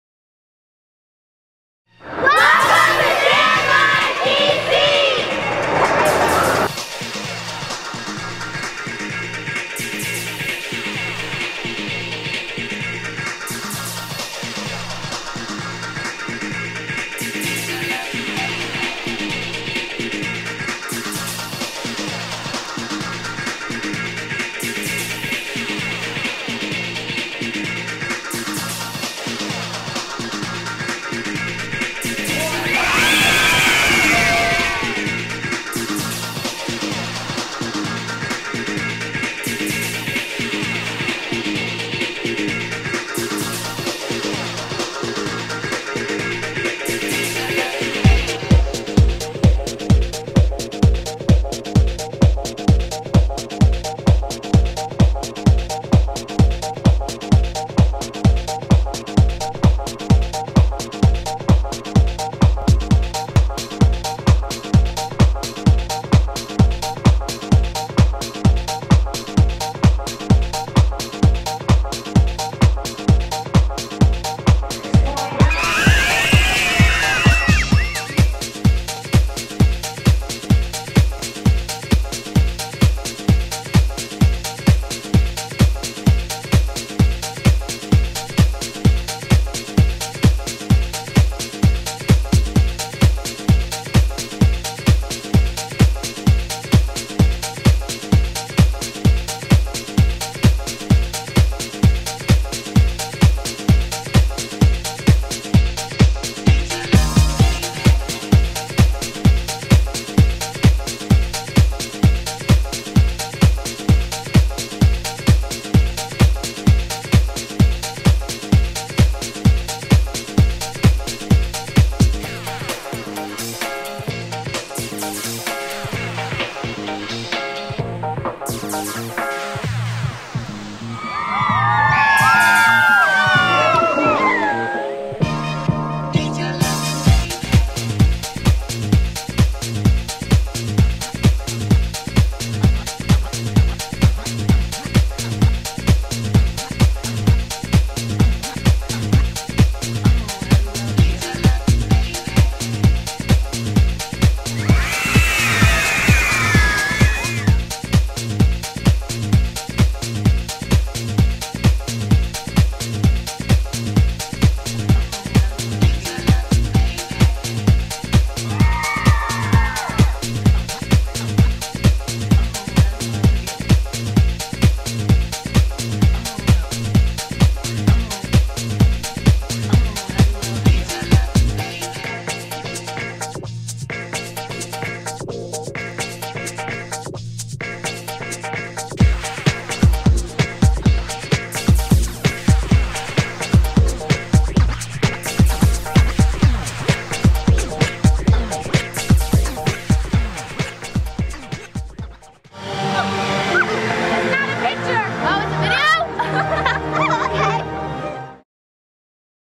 LIVE! Washington DC Red Carpet Show 2011
LIVE! Cheer and Dance Competition in Washington DC January 8, 2011.